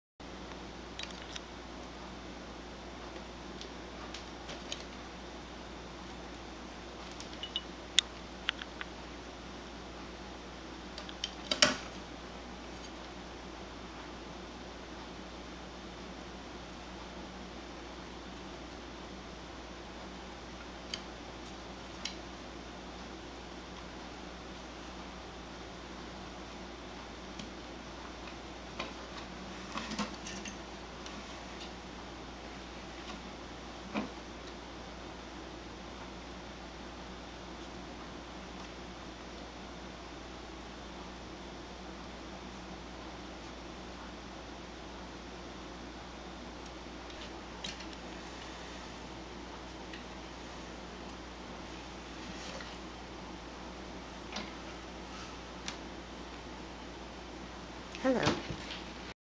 Boo! I Found You!
Kitty Cat hiding in drawer